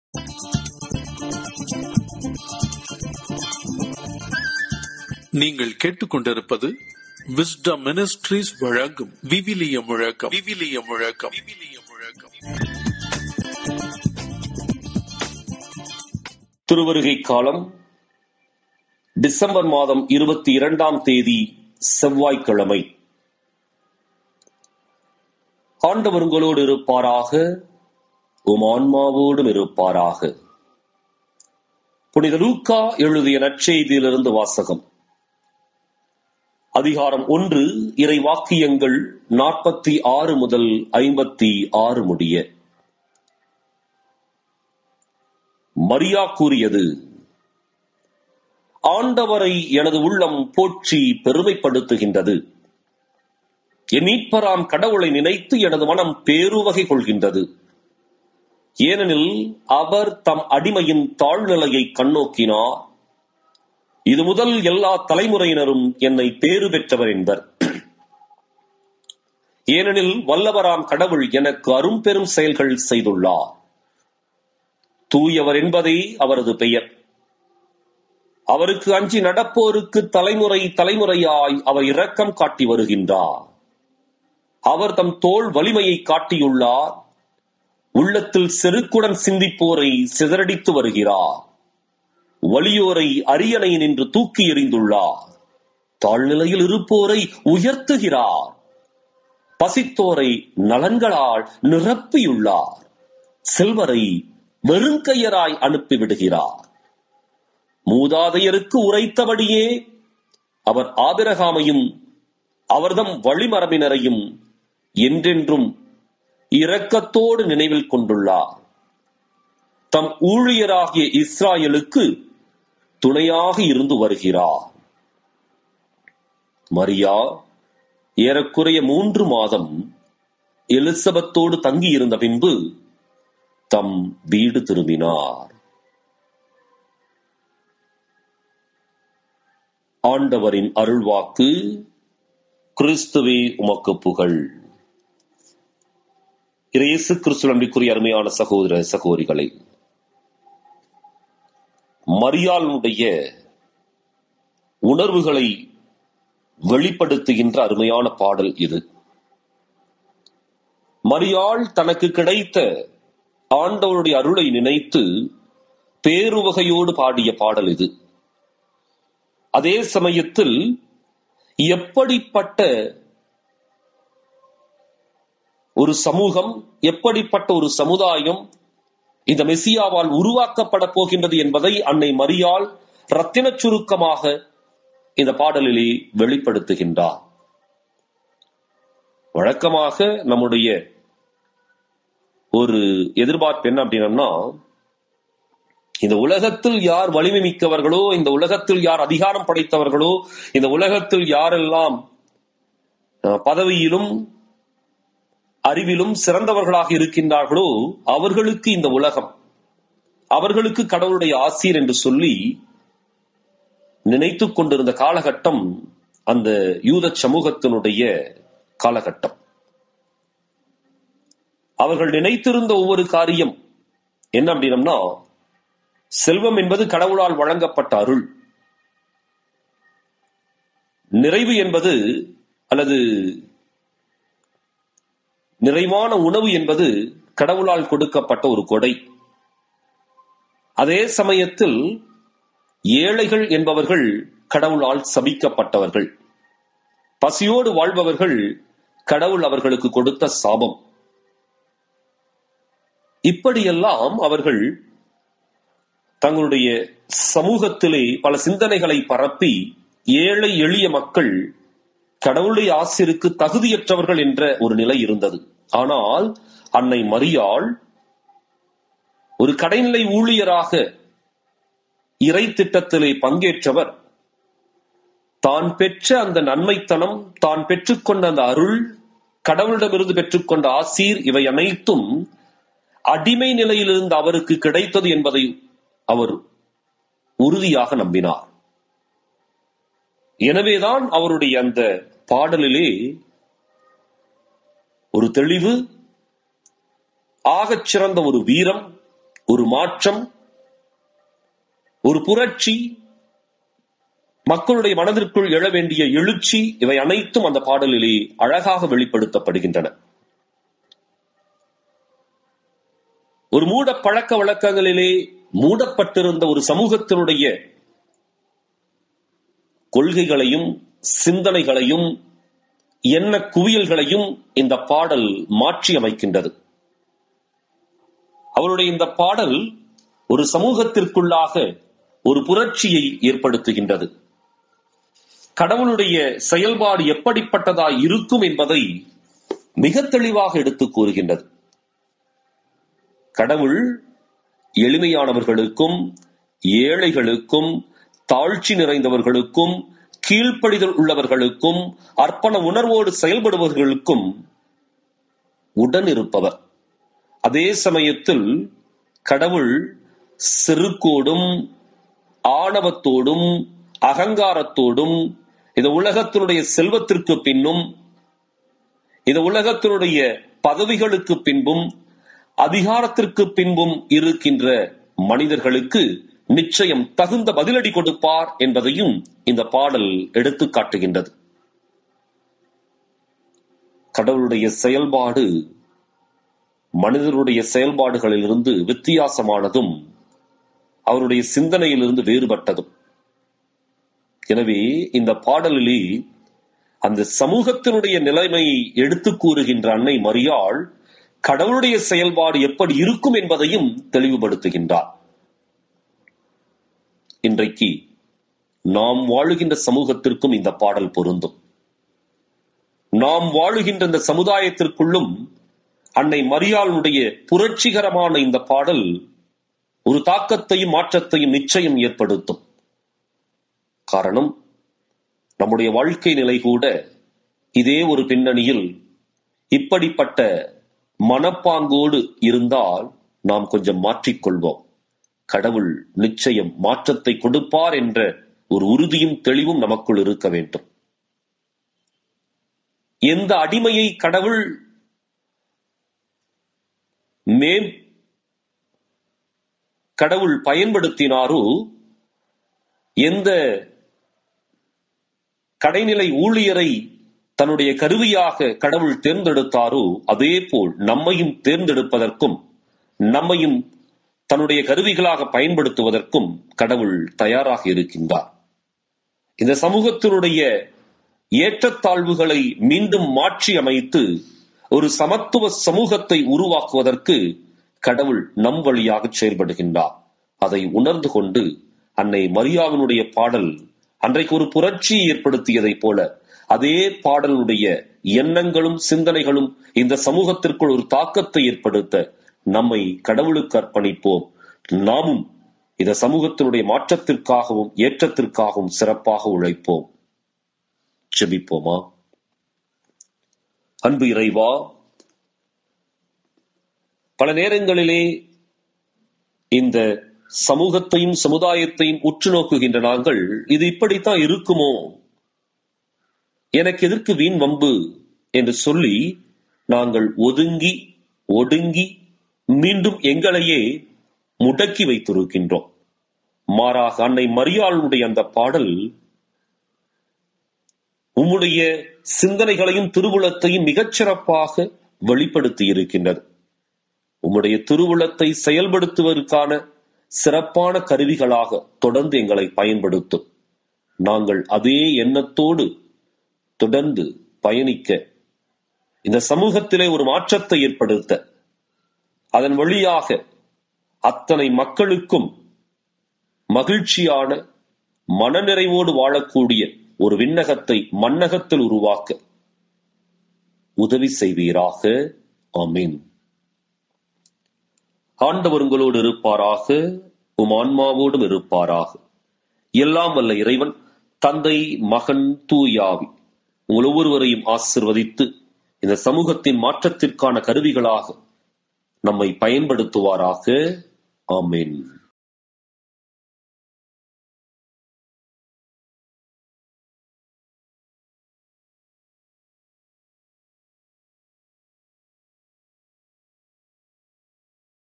விவிலிய முழக்கம் -22.12.2020-Tuesday#Advent-Year B-Week 4#Luke 1:46 56
நல்ல உண்மையான கீழ்படிதலுள்ள நேர்மையான நன்றியுள்ள இதயம் உடையவராய் அன்னை மரியாள் இருக்கின்றார்